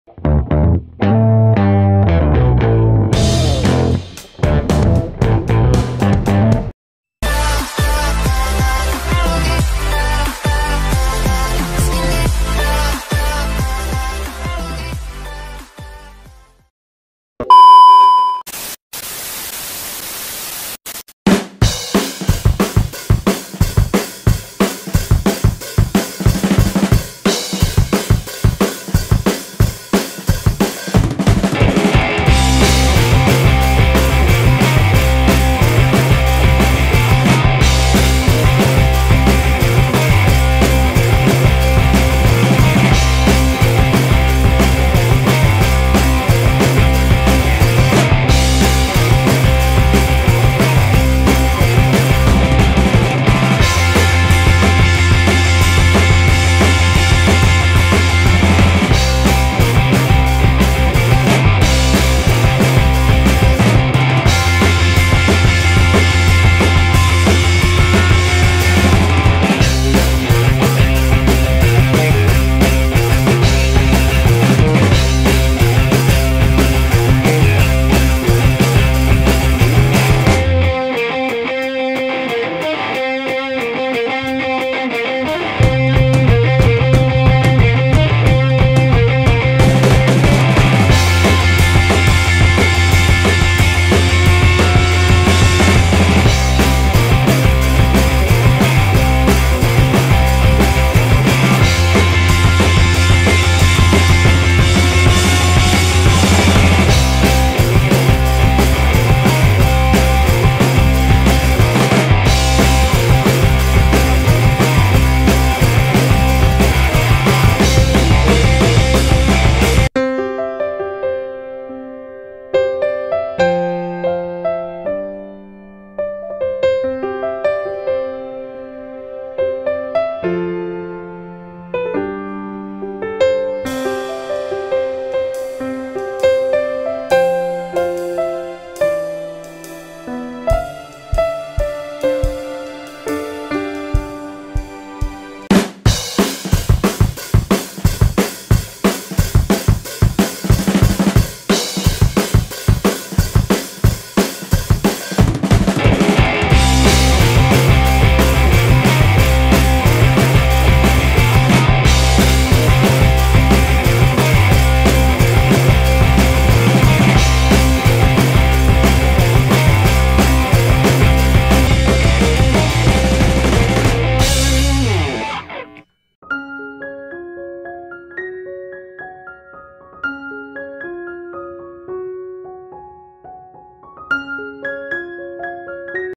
Skill Dewa Ngarit Padi | Mesin Aja Kalah ‼️
Moment ini hanya bisa di lihat satu tahun dua kali, petani di Indramayu Jawa Barat Mempunyai skill dewa dalam mengarit padi dengan menggunakan parang (arit) mau tau cuplikan videonya let's.. go..!!